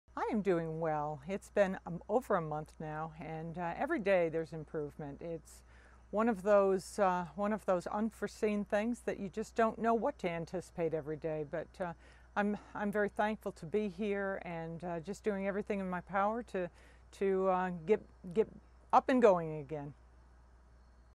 VA TV SHOOTING VICTIM/GARDNER-I"M DOING WELL
Virginia shooting victim says she is doing well now.